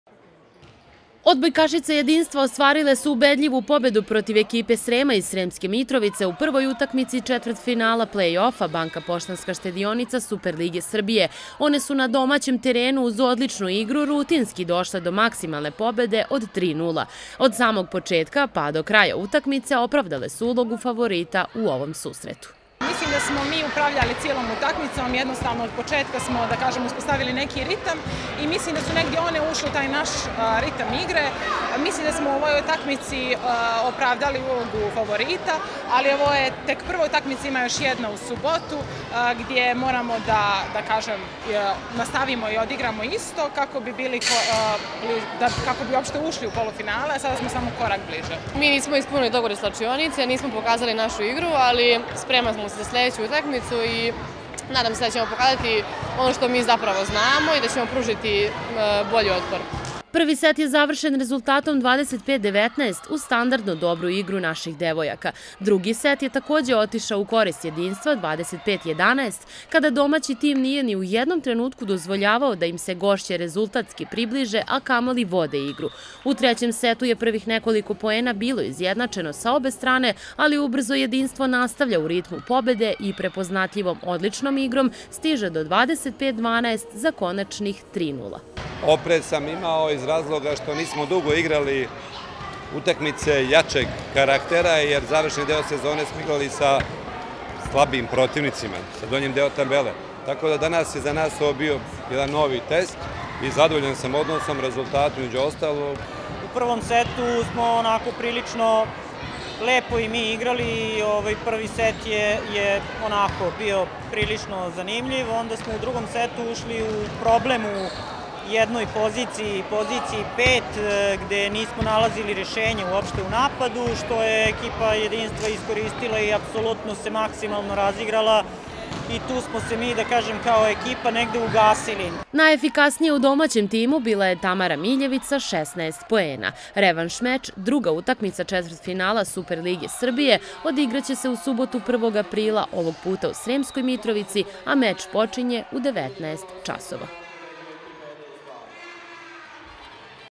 Jedinstvo pobedilo u prvoj utakmici četvrtfinala play offa Superlige
Odbojkaški klub Jedinstvo došao je do druge utakmice 1/4 finala play off-a Superlige sinoćnjom pobedom nad ekipom Srema. Maksimalan rezultat od 3:0 je bio očekivan obzirom na kvalitet staropazovčanki ali ostaje i revanš meč u Sremskoj Mitrovici koji vodi do polufinala, čemu se Jedinstvo iskreno nada.